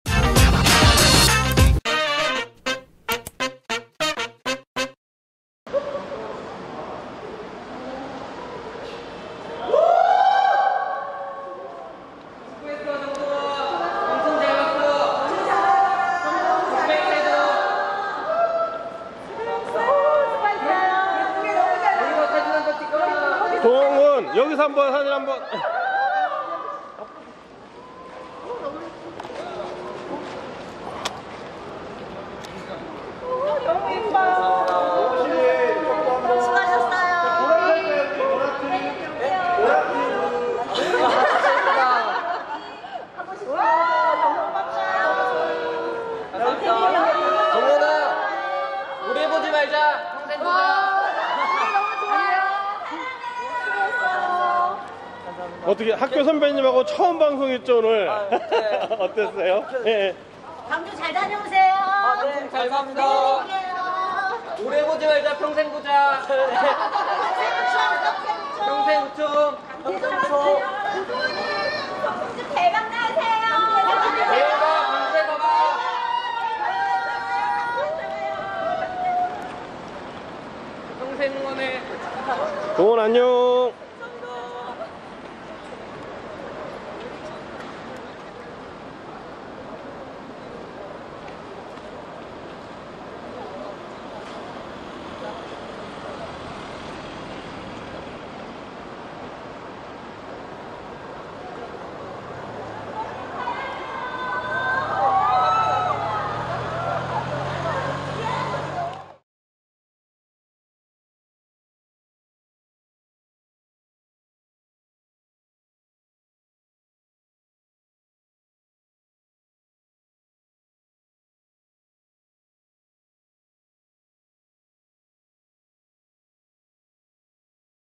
정동원, 미니인터뷰 학교 선배와 첫 방송 소감은? (KBS '아침마당' 퇴근길)
[뉴스인스타 양철수기자]  9월 22일(월) 오전, 서울시 영등포구 여의도 KBS에서 진행된 KBS 1TV ‘아침마당' 생방송 출연을 마치고 가수 정동원이 방송국을 나서며 포즈를 취하고 있다.

정동원, 아침마당, 우주총동원, 우총, 아침마당, 동그라미, 미스터트롯, 하동왕자, 퇴근길